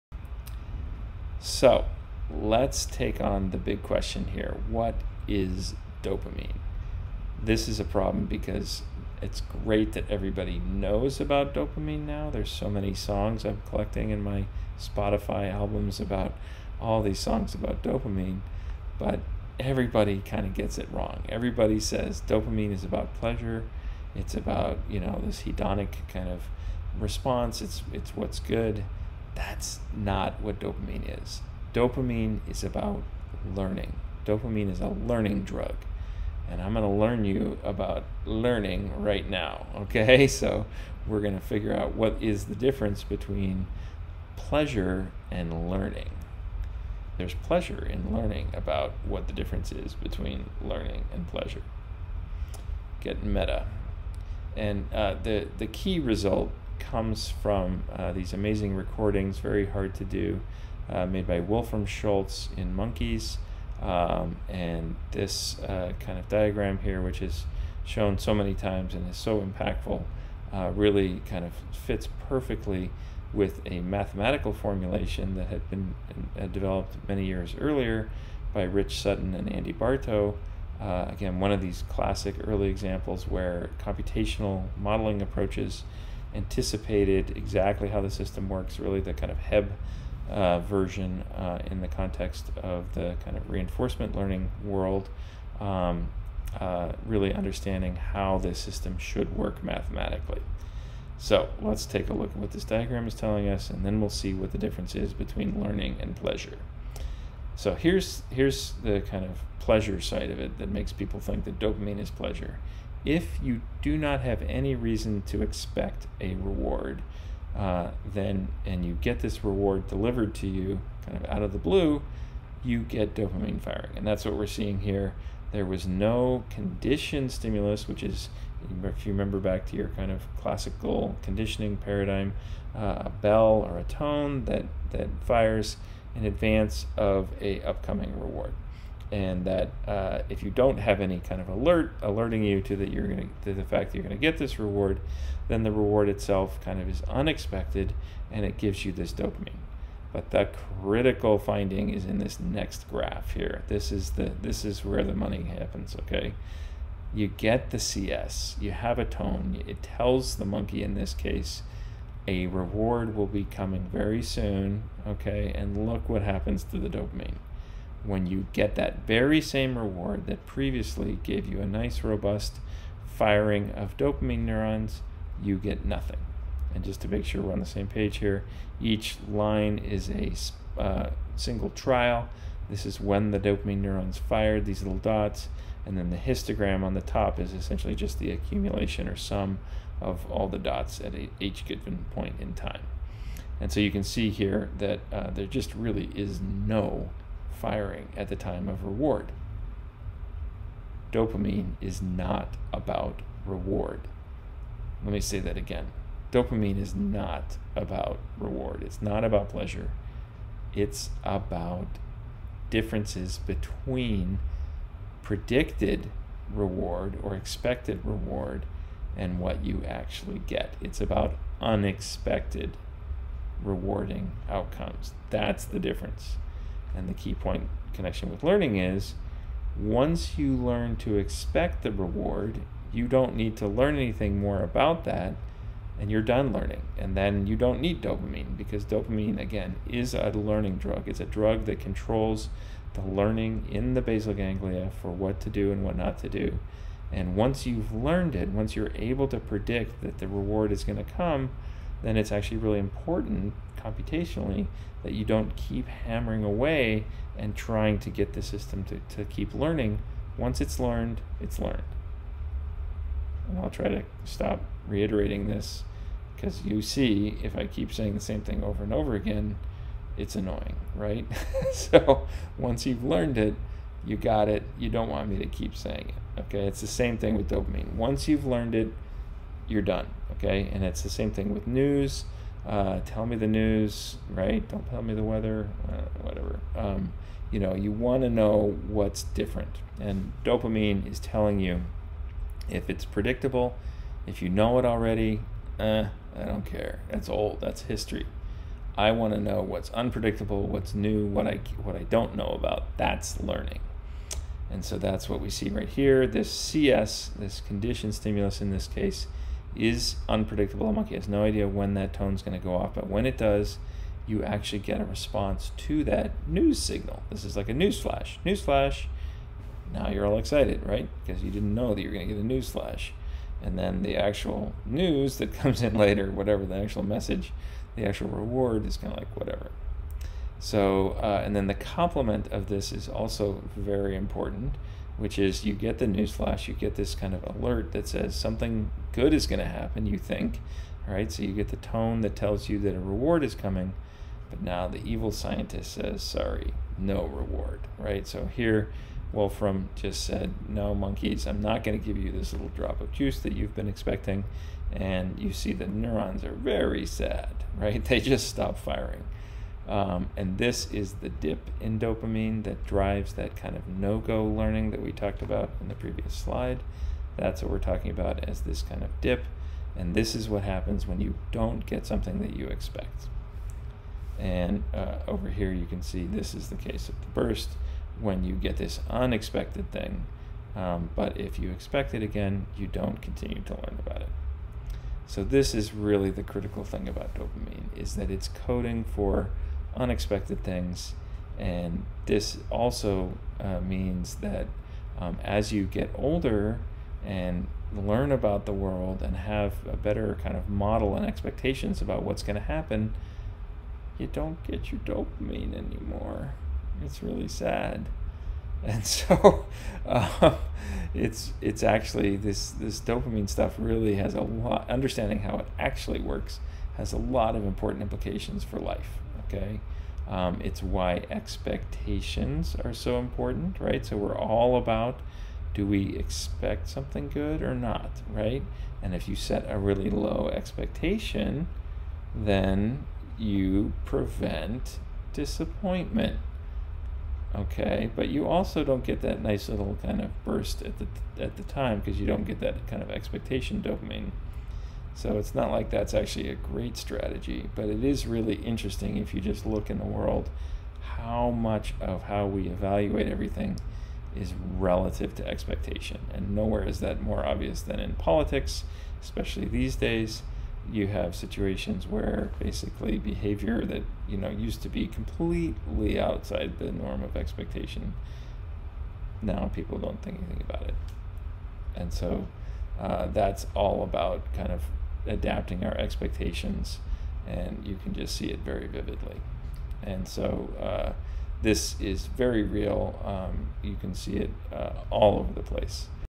CCN Course 2020, Motor 8: Dopamine is reward prediction error
Lecture 8 of 11 for Chapter 7: Motor Control and Reinforcement Learning

Dopamine signals the *difference* between expected and received rewards, not for raw reward or "pleasure", contrary to popular belief.